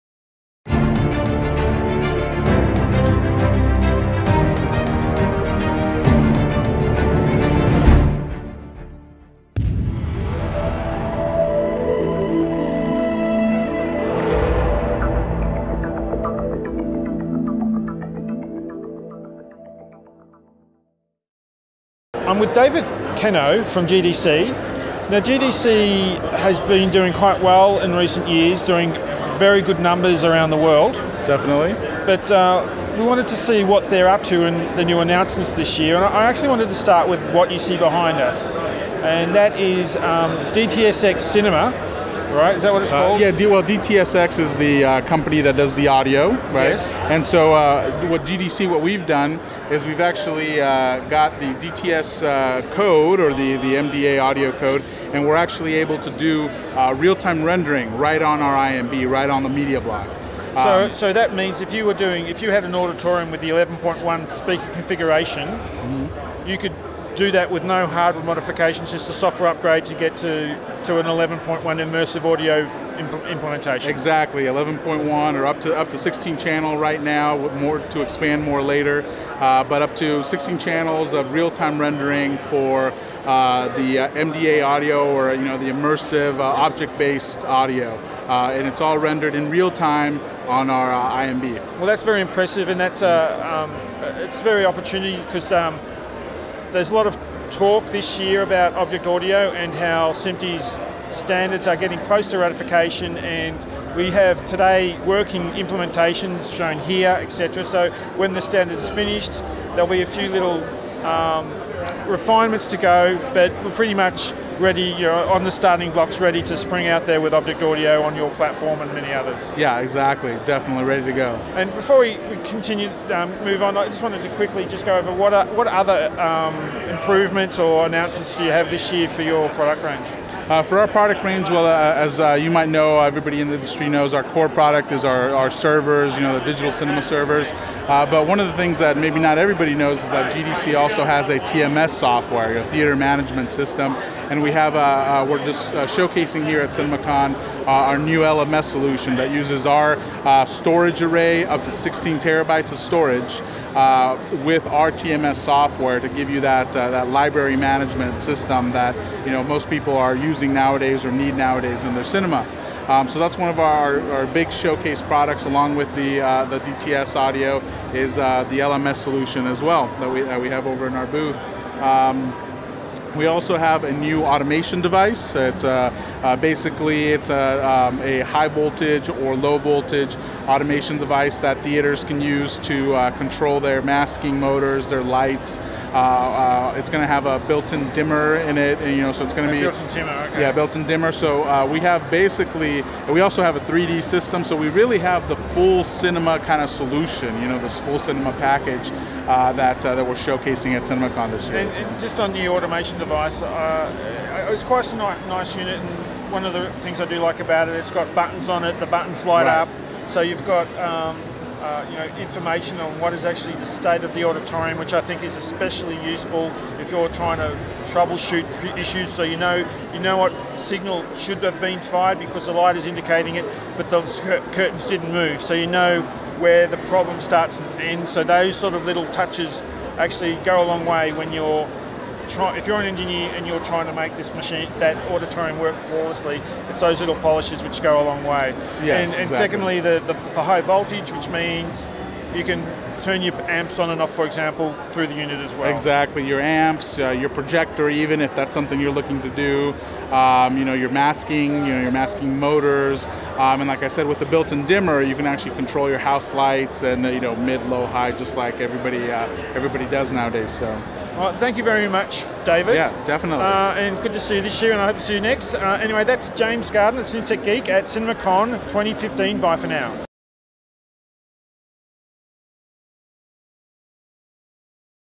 CinemaCon 2015, GDC Product update
David Cano updates us on the latest GDC offerings.  This includes MDA Immersive Audio decoding ON-IMB, no extra audio device needed. (limited to 16 channels), plus a new IO box, TMS features and storage offerings.